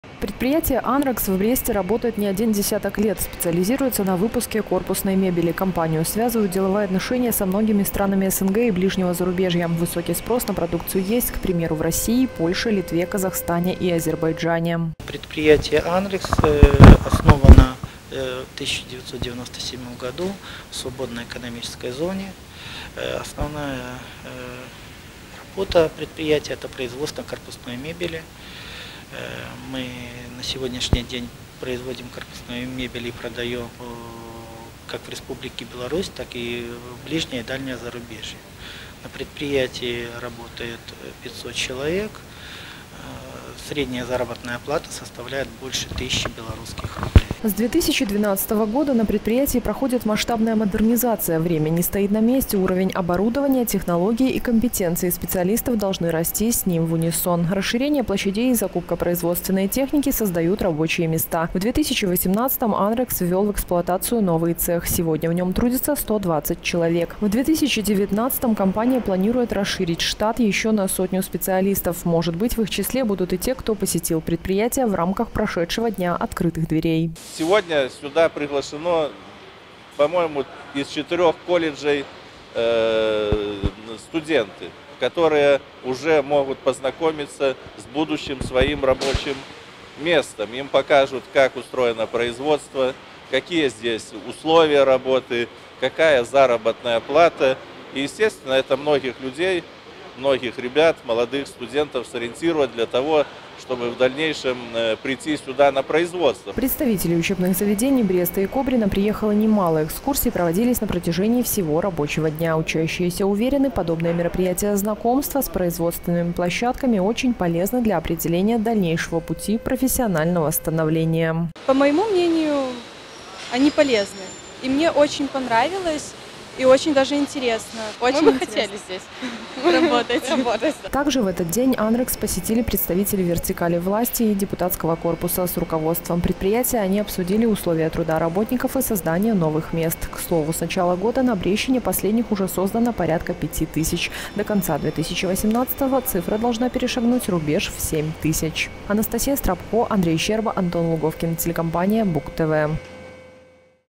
2018-10-30 г. Брест. День открытых дверей на предприятии «АНРЭКС». Новости на Буг-ТВ.
Резидент свободной экономической зоны «Брест» - иностранное общество с ограниченной ответственностью «АНРЭКС» - в октябре провел День открытых дверей. Ознакомиться с производственной базой предприятия, выпускаемой продукцией и познакомиться с руководством компании и коллективом смогли учащиеся Бреста и Кобрина. На мероприятии побывала и съёмочная группа нашего телеканала.